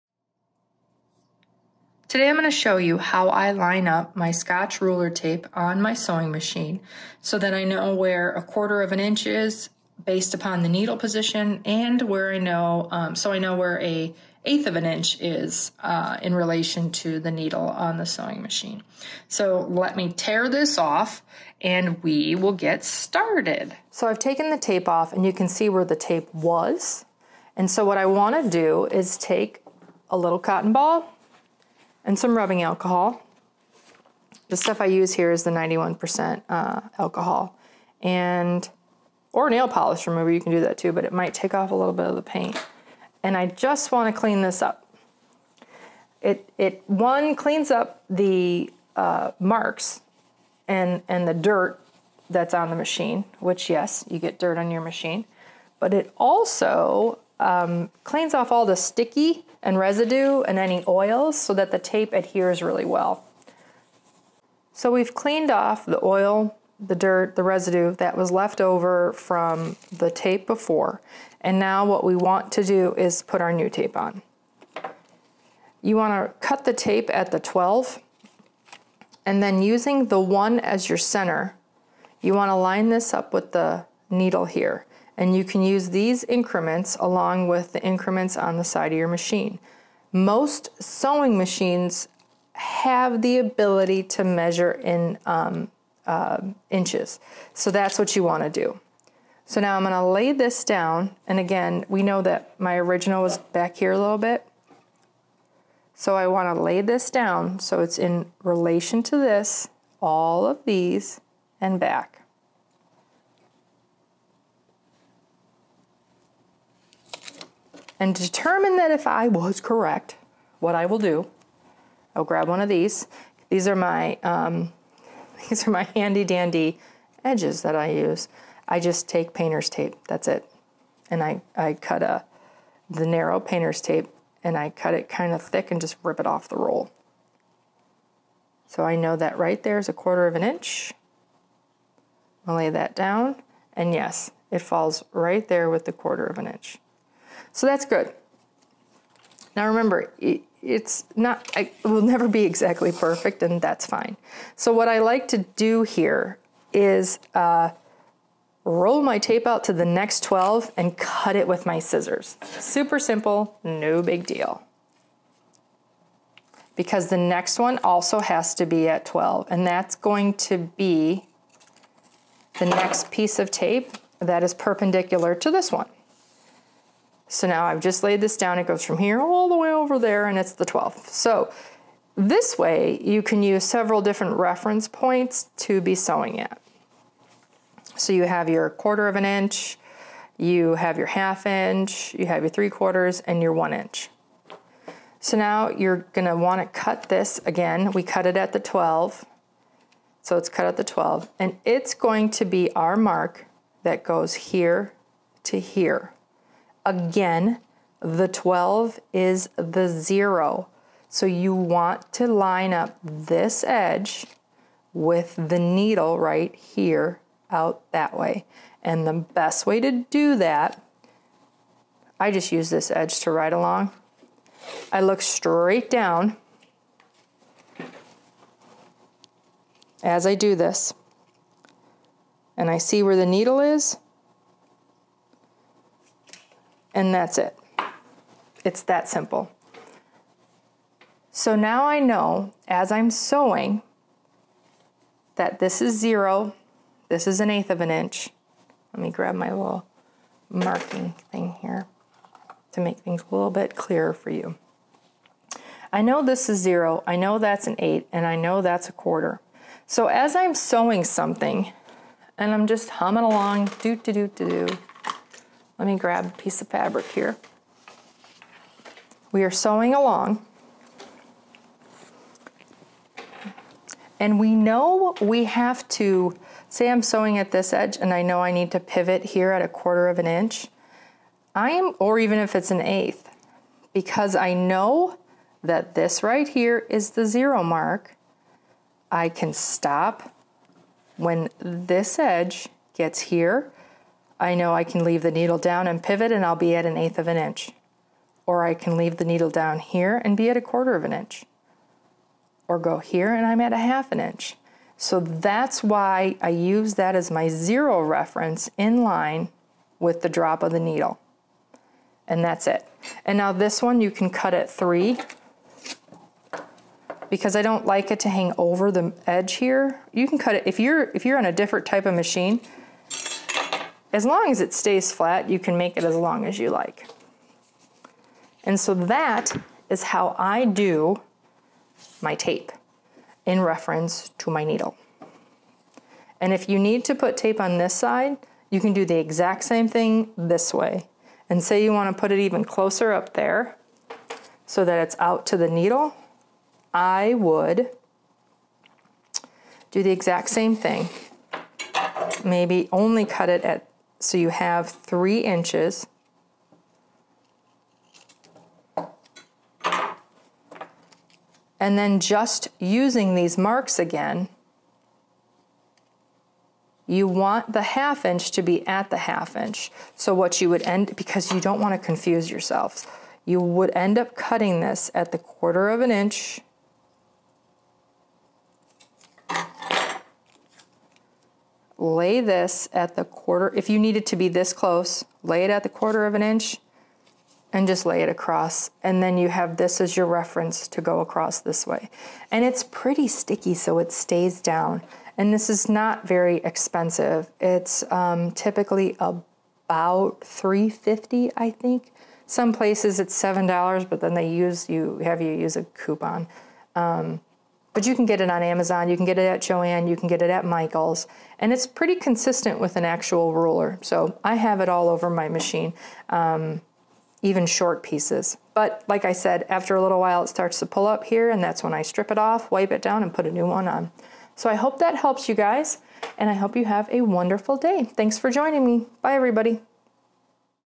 Applying Scotch Ruler Tape
I was asked in my group, Patterns by Amy Lynne, how I add my Ruler Tape to my machine to help guide me when I’m sewingd and I was happy to oblige with a quick video!

The machine I sew on is an industrial straight stitch machine, JUKI DDL 8700. I love it and will never part with her.

I use Scotch Brand Ruler Masking Tape. You can pick it up at most hardware stores and on Amazon and at Michaels. Michaels has terrific same day pick up right now!

It is almost double the cost at Amazon so if you can get it for same day pick up at Michaels you are much better off.

Scotch Brand MASKING TAPE MEASURE .94X20YD, 0.94 in x 20 yds, Multicolor

If you’d like to join my Facebook group please head over we’d love to have you!